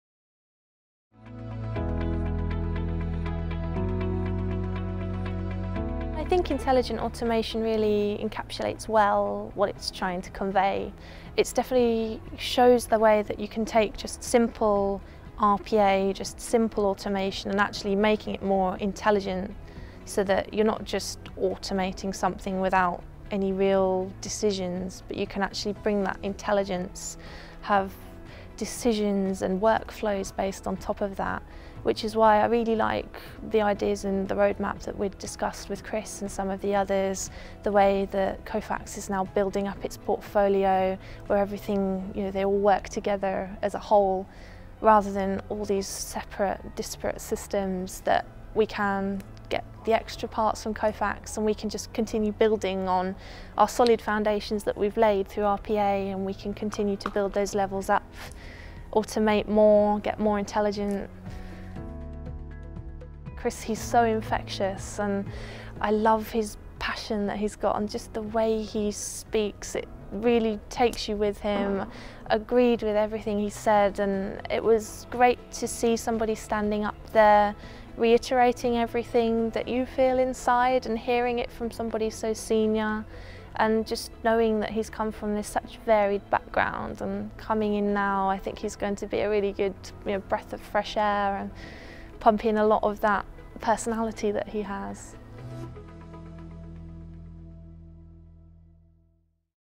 Intelligent Automation Insights: Emily Harper of Stolt Tank Containers
With successful Kofax RPA implementation, Stolt Tank Containers look toward the Kofax Intelligent Automation platform to build their digital workforce to Work Like Tomorrow.